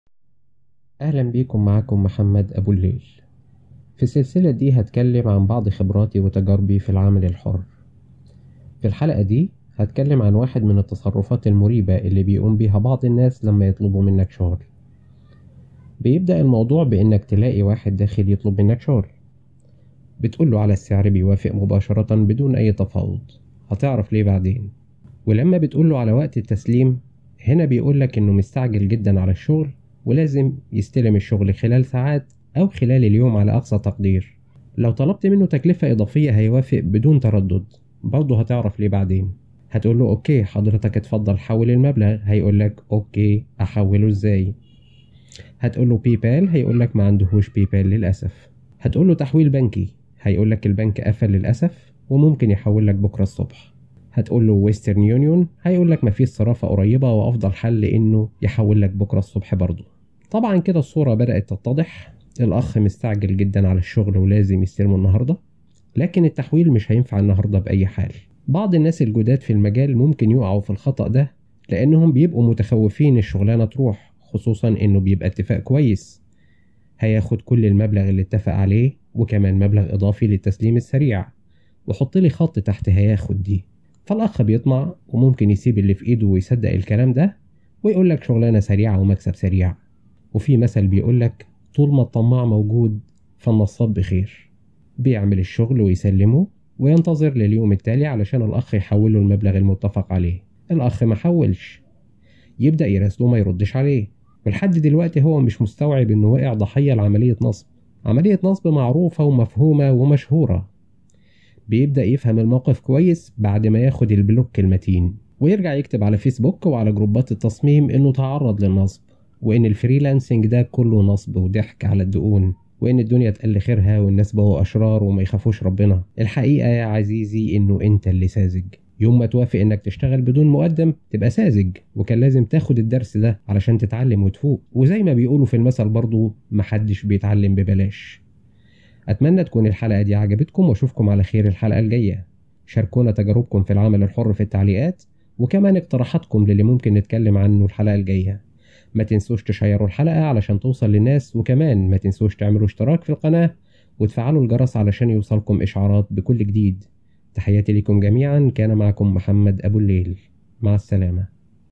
خبراتي في العمل الحر | الحلقة الأولى | الفريلانسينج نصب وضحك ع الدقون
فى هذه الحلقات الصوتية أشارك معكم خبراتى في العمل الحر ( الفريلانسينج ) بشكل مختصر وسريع
أتكلم عن العمل من خلال الإنترنت عموما والصعوبات التى تواجه الفريلانسر وكيف يواجهها